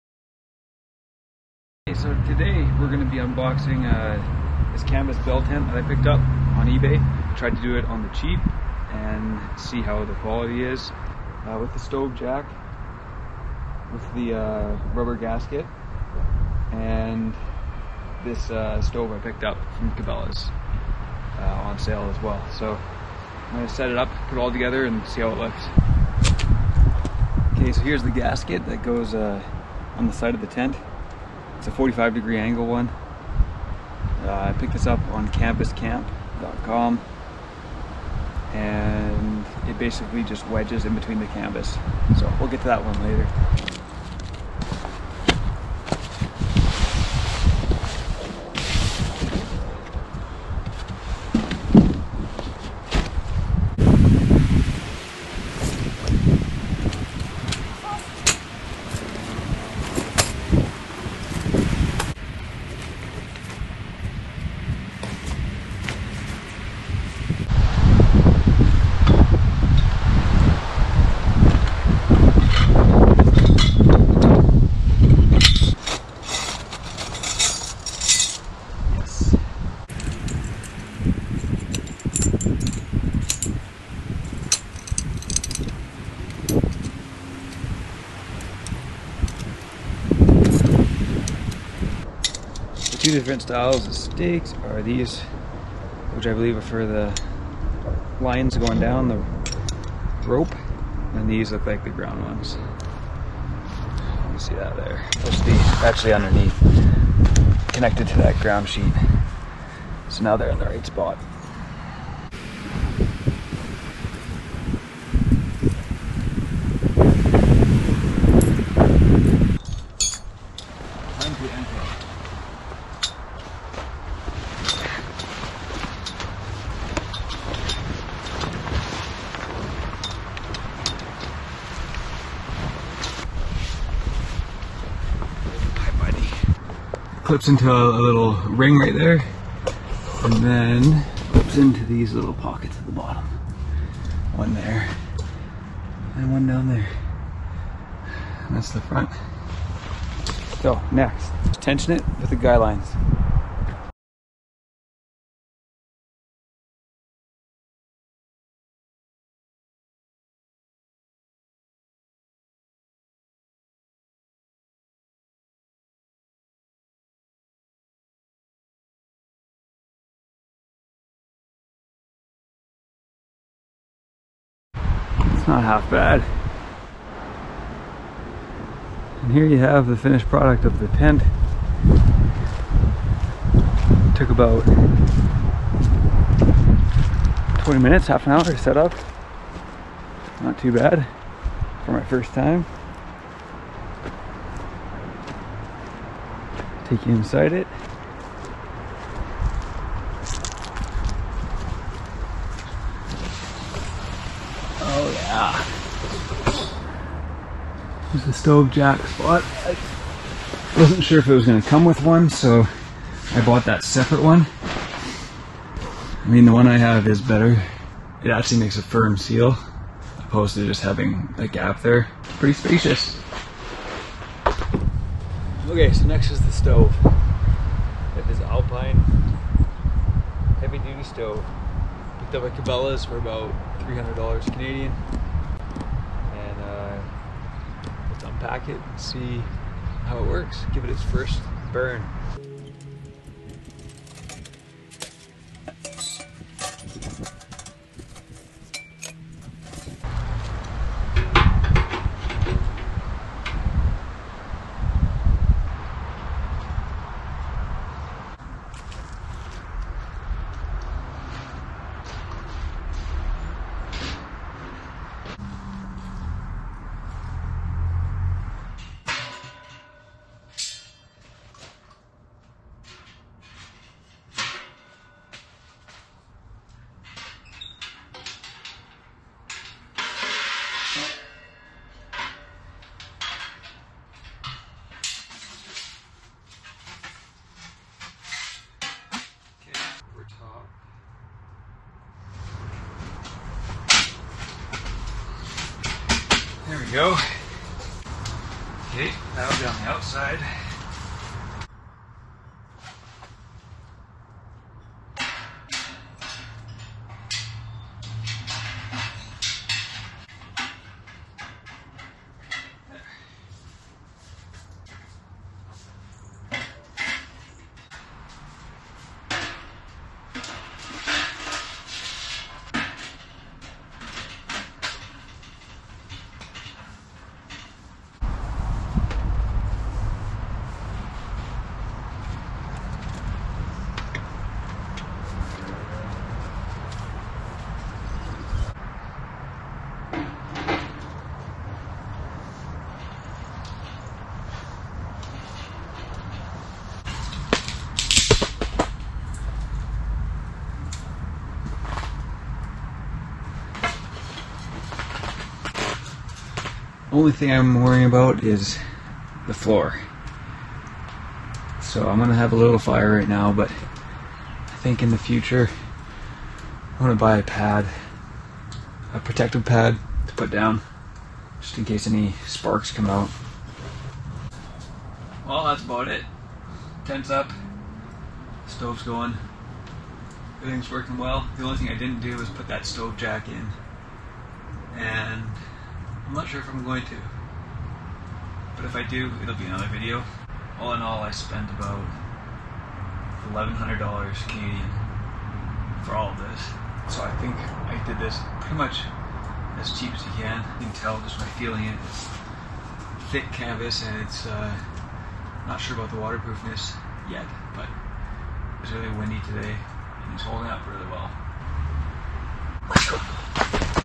Canvas Bell Tent Setup
Setting up my 4-meter canvas bell tent. I tried getting the full setup for as cheap as possible by shopping on eBay for the tent and getting the stove from Cabelas on sale. In the end, it cost me around $1100 Canadian. I have used it many times since this video and it has been perfect.